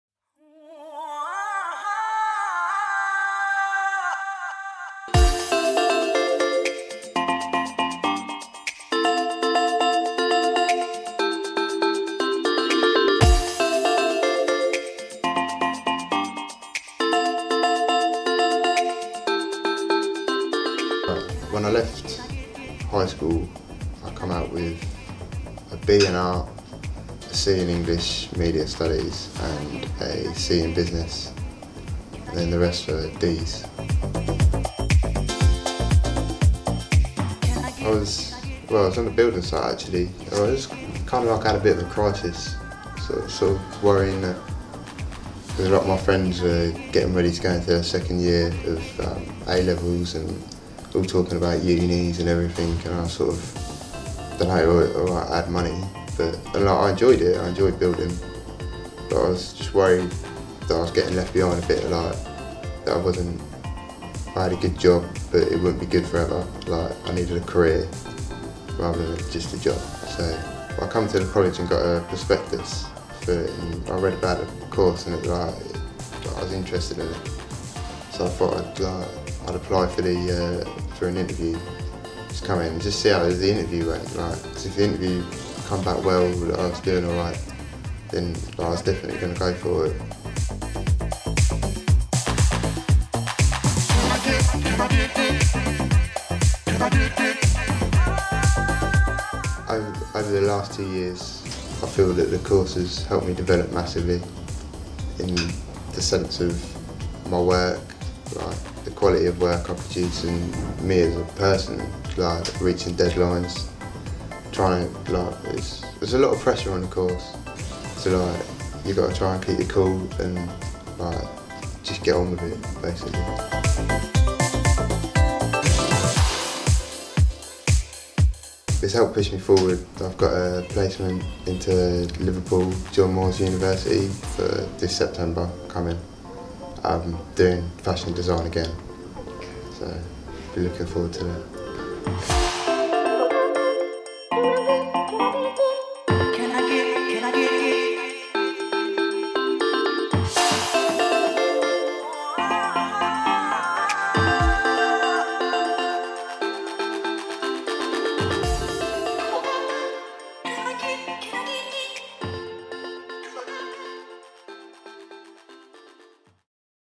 Northampton College student - builder turns fashion guru
Tom Yeo used to be a builder before opting for a career in fashion!